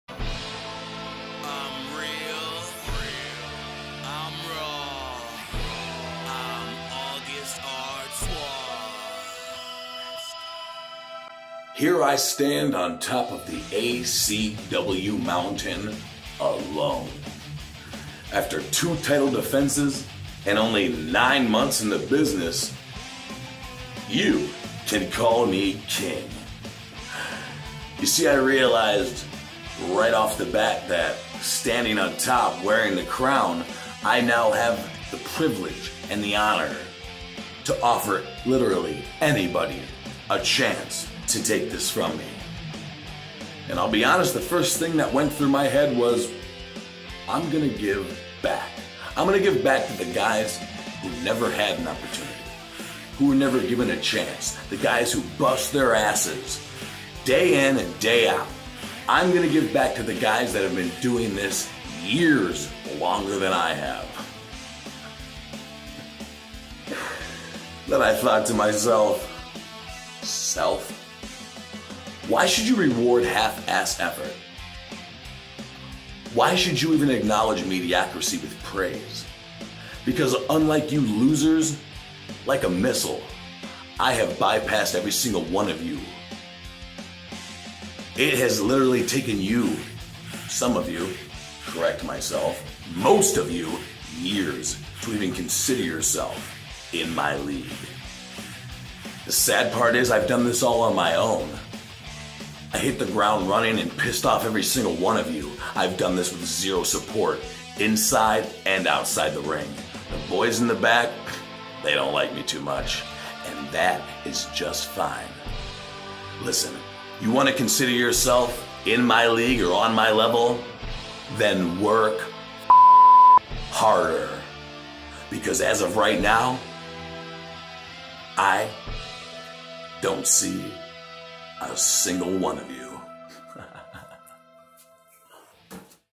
Message To: The Overlooked From: August Artois
This message is for the guys that have been busting their ass in this business for years and have been told that they are nothing more than "a good hand".

This message is for the guys that were never given a shot.

This message is for the guys that were never believed in.

This message is for the hungry.

WWNLive x ACW - American Combat Wrestling

August Artois 🔥